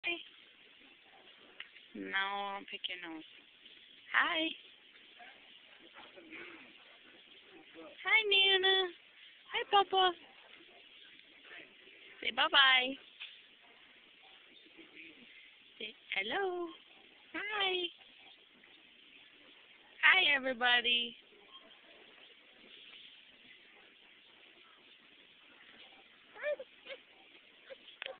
Gavin waving!
My mommy got a video in Walmart of Gavin waving. :D So cute.